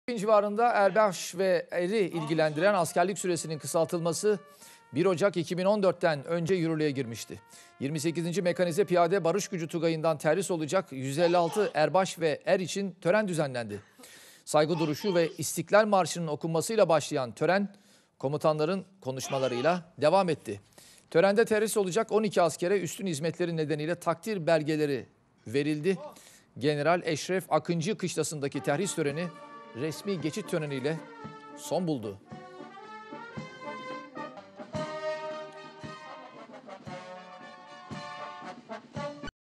ASKERE YENİ YIL HEDİYESİ
KIŞLALARDA ERKEN TERHİS HEYECANI YAŞANMAYA DEVAM EDİYOR... ASKERLİK SÜRESİNİN 15 AYDAN 12 AYA İNDİRİLMESİNİN ARDINDAN, ANKARA'DA 156 ERBAŞ VE ER İÇİN TERHİS TÖRENİ DÜZENLENDİ...